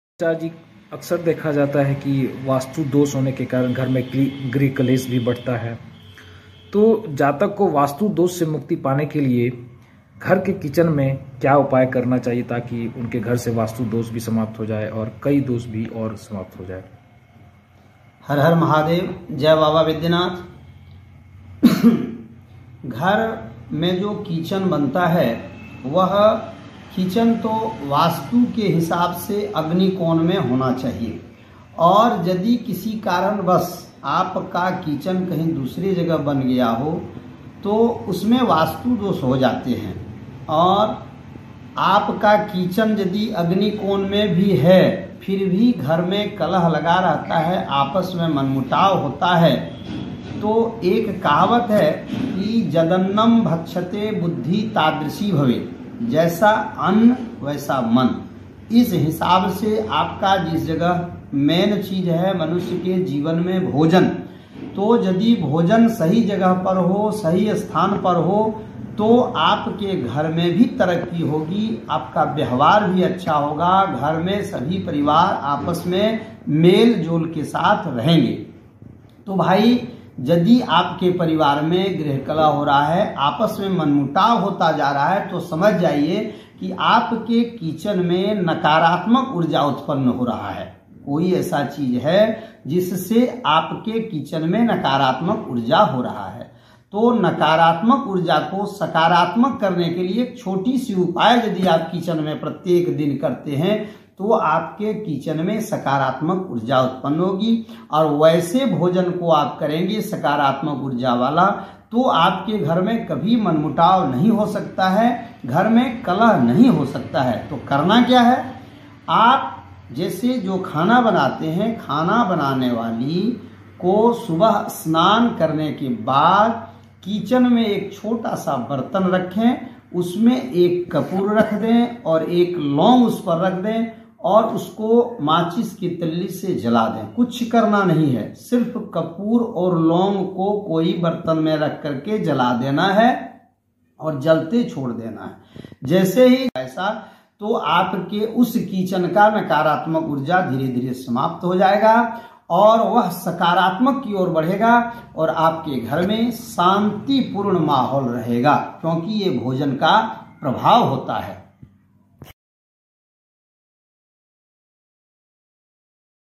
Vastu Tips : बड़े खतरनाक होते हैं किचन से जुड़े वास्तु दोष दूर करने के लिए करें ये उपाय
Pandit Nand Kishore Mudgal, astrologer of Deoghar, said that if there is a Vastu defect in the kitchen of the house, it also makes the food bad and the home affliction also increases in the house. You can get rid of these defects only by home remedies.

देवघर के ज्योतिषआचार्य पंडित नंदकिशोर मुद्गल ने जानकारी देते हुए कहा कि अगर घर के किचन में वास्तु दोष है इसे खाना भी खराब बनता है और घर में गृह क्लेश भी बढ़ता है. घरेलू उपाय से ही इन दोषों से पा सकते हैं मुक्ति.